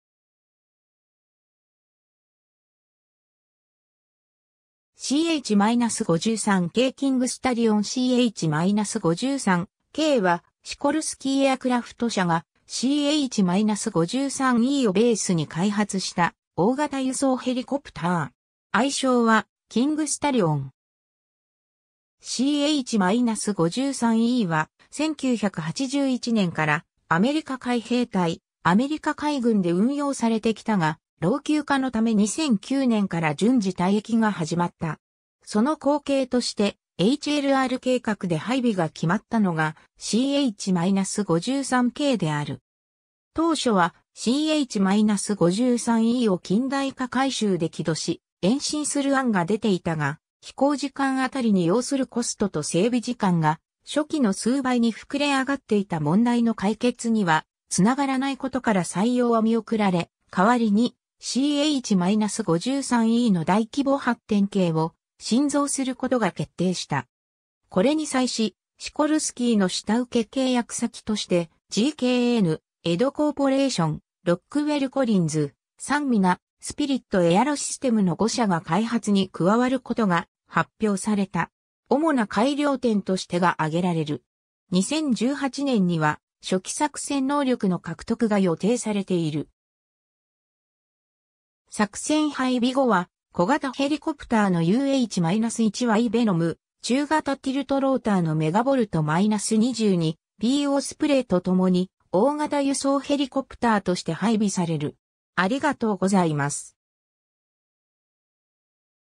CH-53K (航空機)
CH-53K キングスタリオン CH-53Kは、シコルスキー・エアクラフト社がCH-53Eをベースに開発した大型輸送ヘリコプター。
愛称はキングスタリオン（King Stallion）。
 CH-53Eは、1981年からアメリカ海兵隊・アメリカ海軍で運用されてきたが、老朽化のため2009年から順次退役が始まった。
その後継としてHLR（Heavy Lift Replacement）計画で配備が決まったのがCH-53Kである。
 当初はCH-53Eを近代化改修で機齢延伸する案が出ていたが、飛行時間あたりに要するコストと整備時間が初期の数倍に膨れ上がっていた問題の解決にはつながらないことから採用は見送られ、代わりにCH-53Eの大規模発展型を新造する事が決定した。
これに際し、シコルスキーの下請け契約先として、GKN、EDO Corporation、ロックウェル・コリンズ（ロックウェル・インターナショナルの後身会社の1つ）、Sanmina、スピリット・エアロシステムの5社が開発に加わる事が発表された。
 主な改良点として が挙げられる。
 2018年には初期作戦能力の獲得が予定されている。
作戦配備後は、小型ヘリコプターのUH-1Y ヴェノム、中型ティルトローターのMV-22B オスプレイと共に、大型輸送ヘリコプターとして配備される。